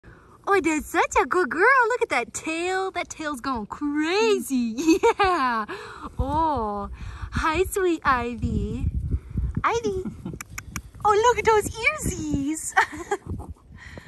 Bowtie Pomsky - Baby Ivy!
Awwww! IVY! That little tail wag just melts my heart! I am in LOVE!